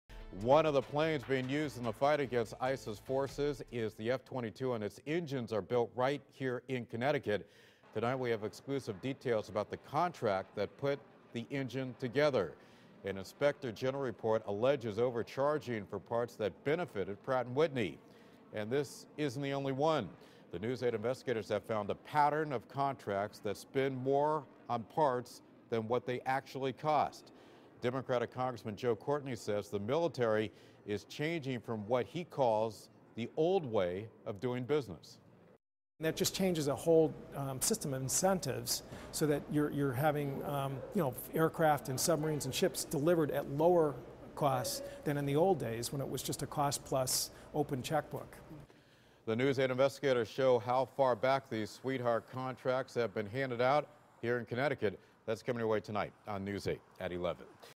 News 8 Investigators: Pratt & Whitney charged multiple prices for same F-119 engine parts
A Department of Defense Inspector General report alleges Pratt & Whitney did not validate actual costs for spare parts for the F-119 engine, used in the F-22 Raptor.